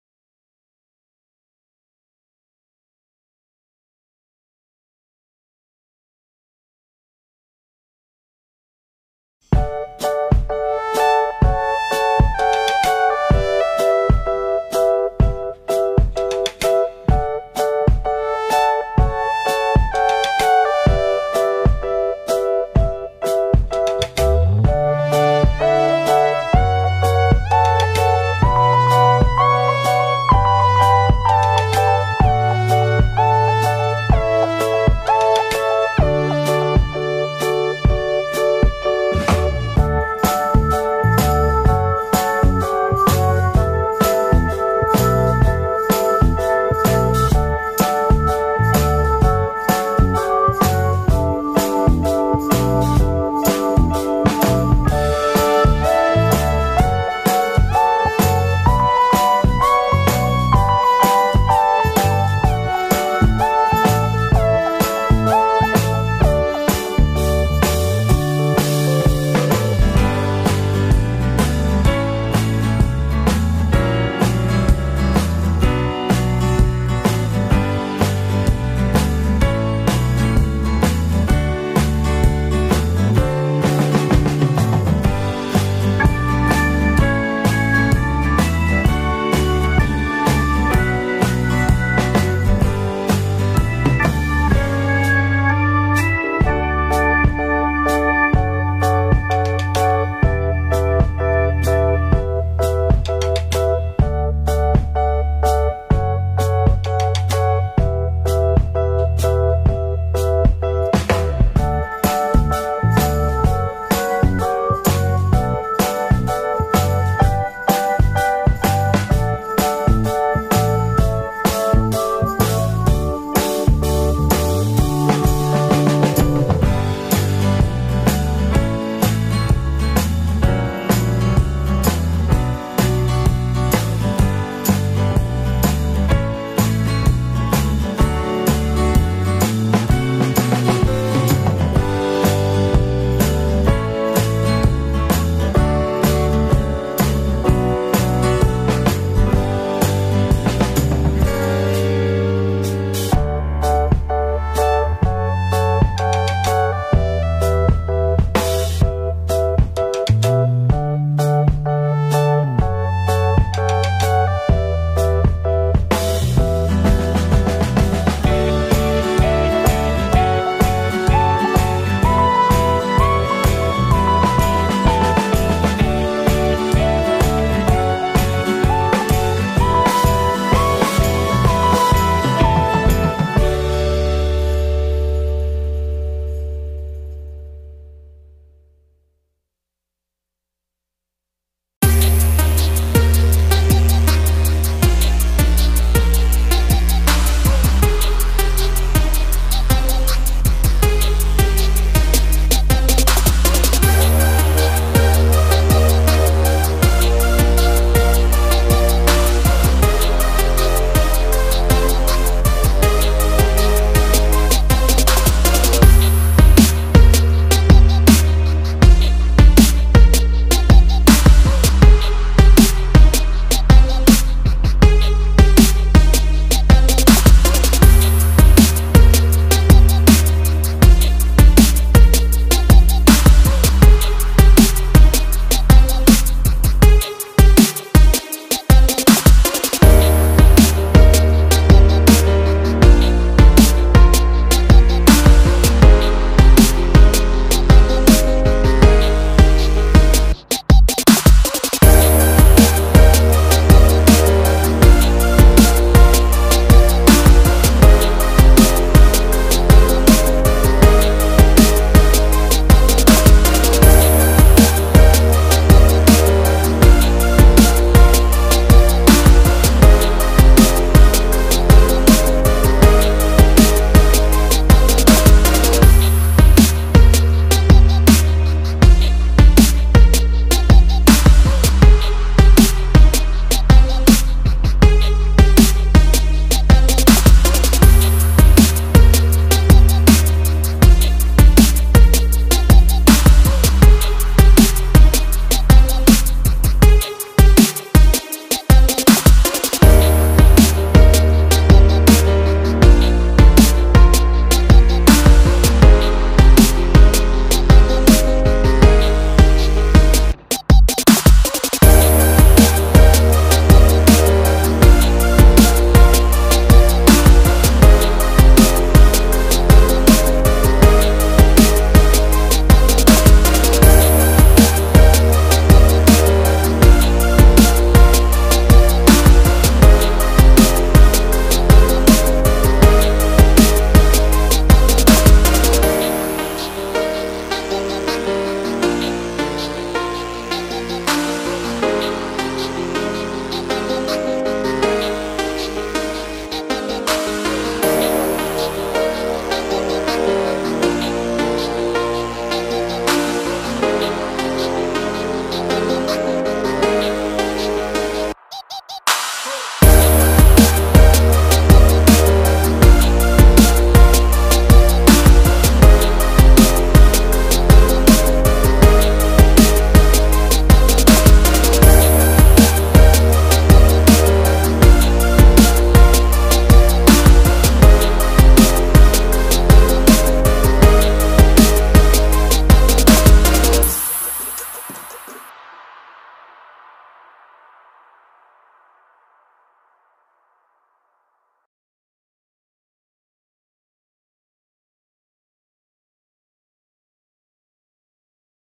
CARA MEMBUAT SUPER TAMIYA KERTAS
super tamiya kertas di buat menggunakan kertas HVS roda dari kardus dinamo batre saklar. hanya sekedar hiburan.